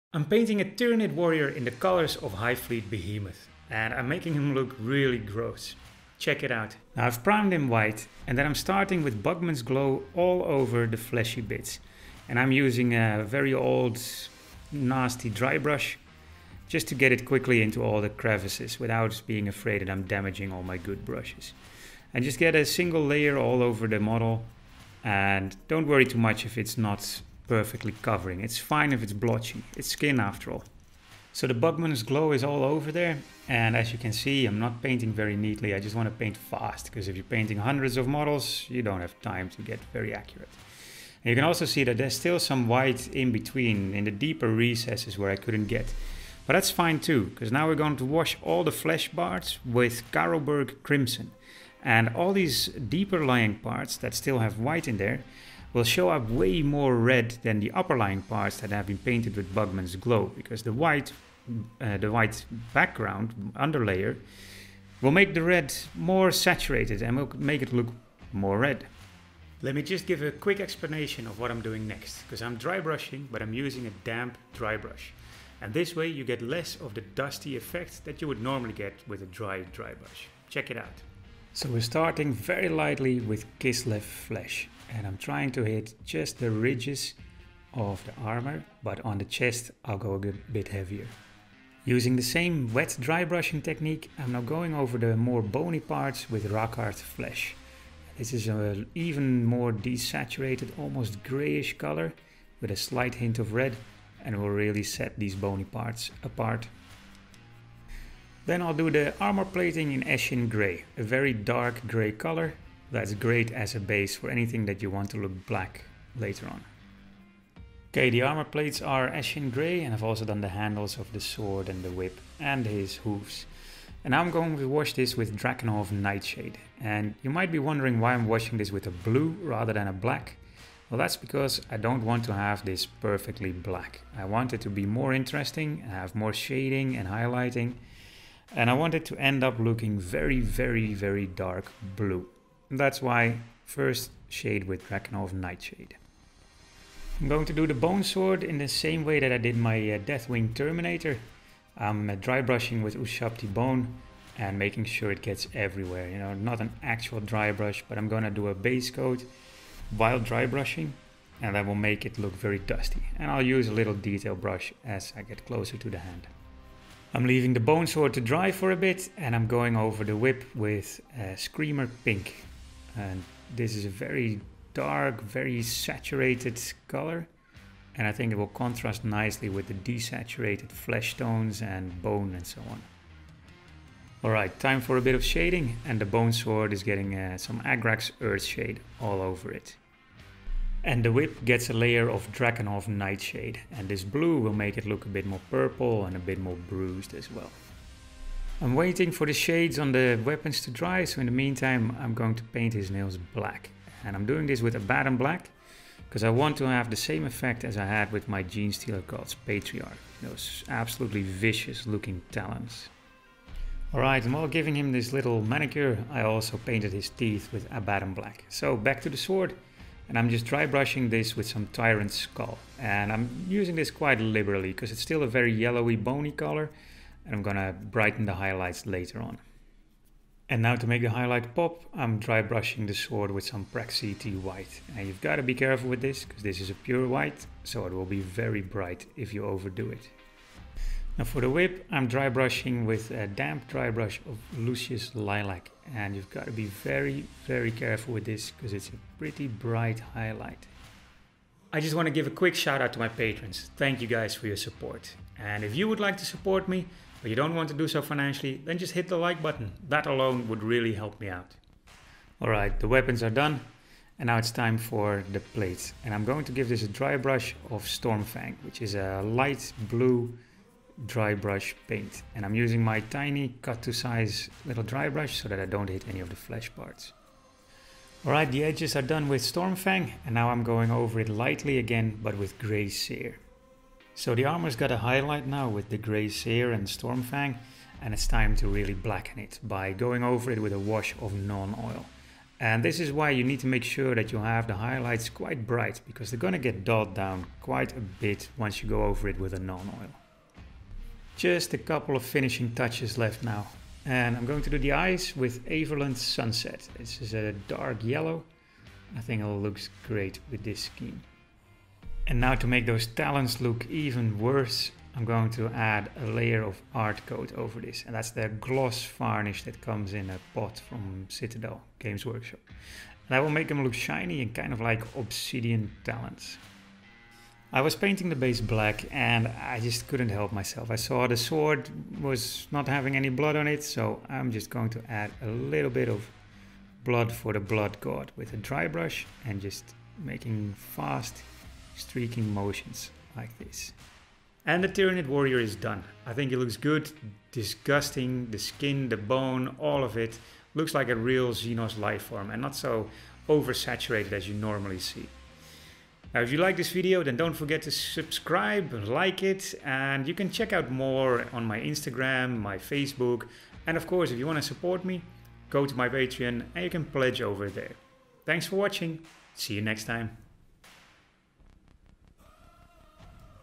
How to paint Tyranid hive fleet Behemoth in a simple grimdark style
If you are a beginner miniature painter and you want to have a great looking Tyranid army, then this might be the scheme for you. It's relatively easy to learn and it is really fast to paint on the few hundred models that you probably want to get in the end if you're playing Tyranids.

You can get a lot done with just drybrushing, washing and dome detail work and you can slo adjust this paint recipe for the smaller models in the army so that you spend even less time on them.

Paints used:

    Bugman's Glow
    Carroburgh Crimson
    Kislev Flesh
    Rakarth Flesh
    Eshin Grey
    Drakenhof Nightshade
    Ushabti Bone
    Screamer Pink
    Agrax Earthshade
    Abaddon Black
    Tyrant Skull
    Praxeti White
    Lucius Lilac
    Stormfang
    Grey Seer
    Nuln Oil
    Averland Sunset
    Ardcoat
    Blood for the Blood God

The basing material is from Geek Gaming Scenics. It's the arid scrubland mix.

Timestamps:
0:00 Painting Tyranid hive fleet Behemoth flesh
1:47 Basing the Carapace
2:26 Painting the Weapons
4:10 Highlighting the Carapace
5:01 Detailing
5:45 Turntable